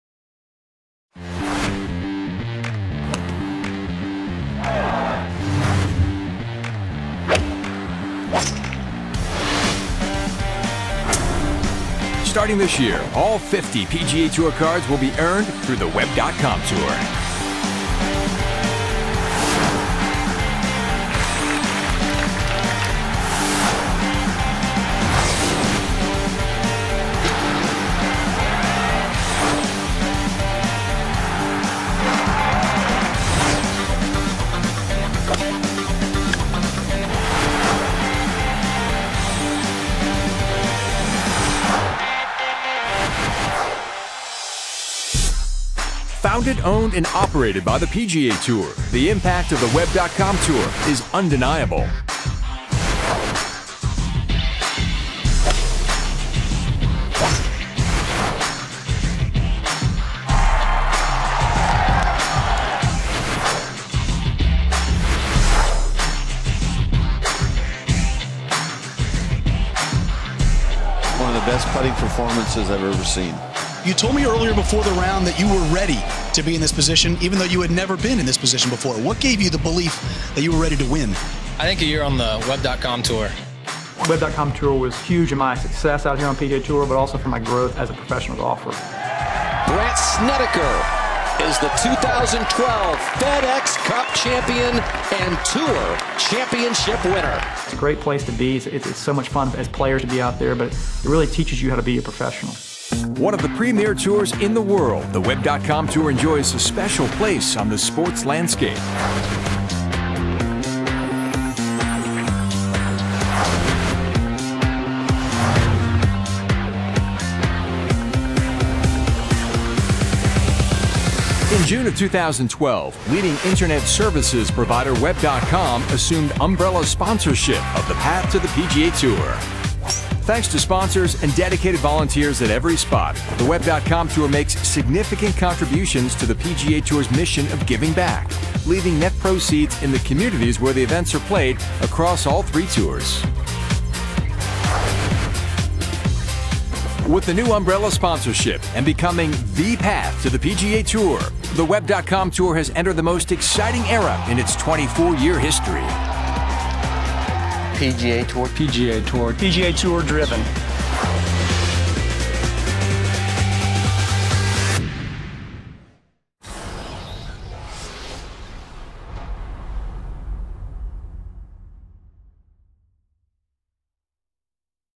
Web.com Tour Ushers in New Era
It's a new era in golf starting in 2013 with all 50 PGA TOUR Cards being earned through the Web.com Tour. .